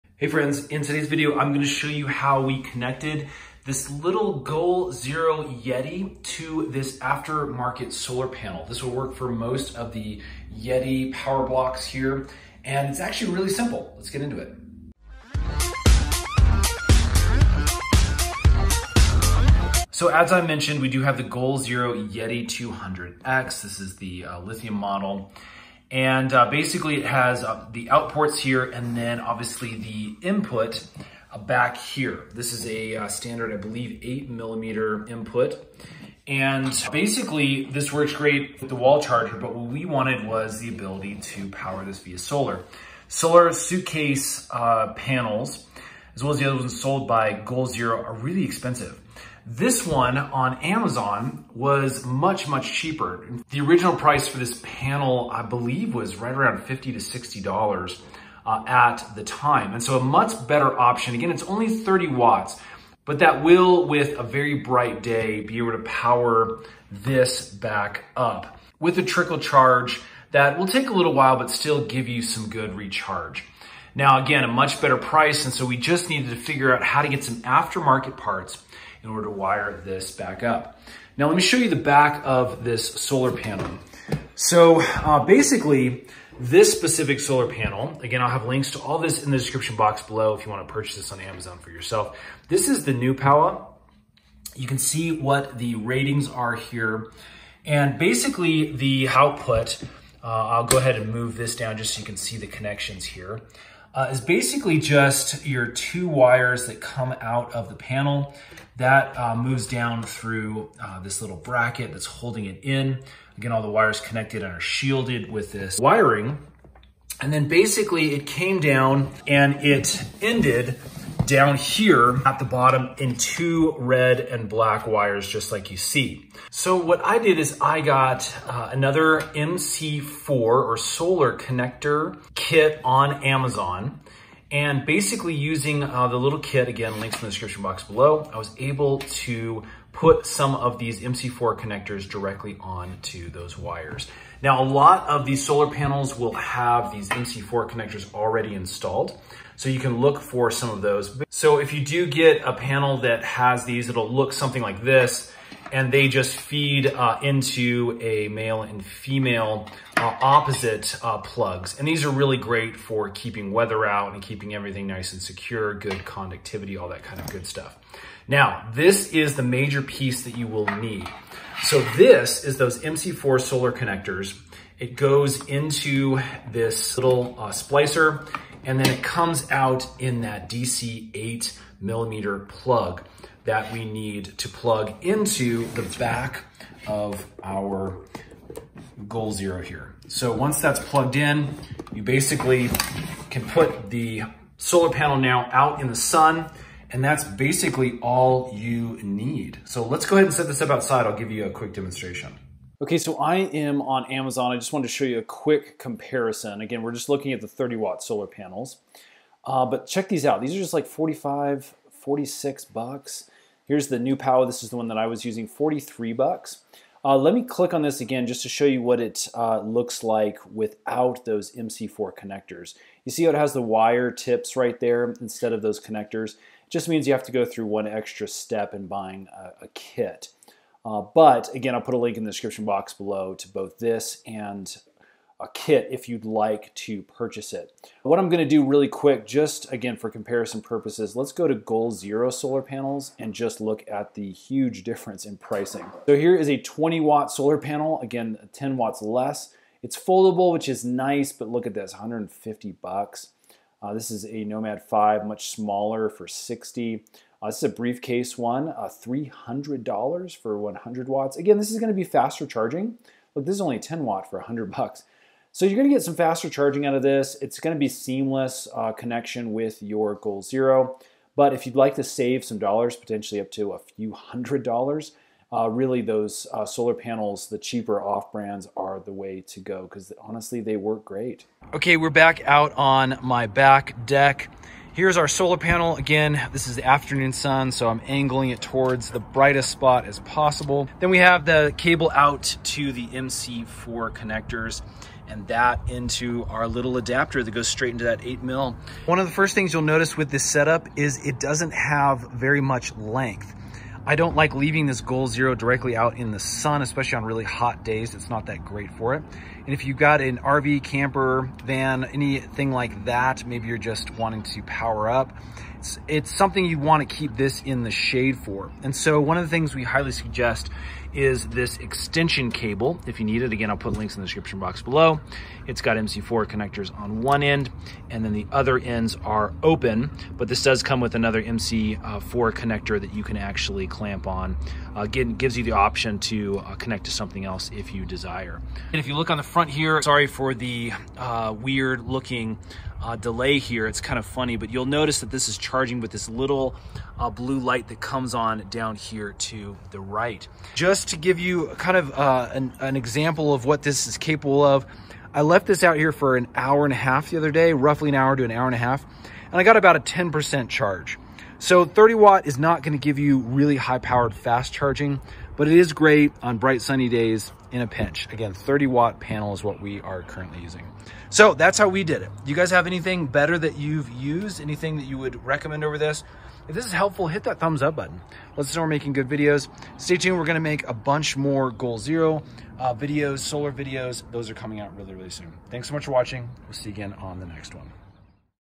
How To Connect Third-Party Solar Panels to Goal Zero Power Station
Buy the parts you need here:
Goal Zero Yeti 200x
Goal Zero MC4 to 8mm Adapter
MC4 Solar Cable Extension
MC4 Solar Connection Kit

In this video, we show you how to charge your goal zero power station with a third- party affordable solar panel.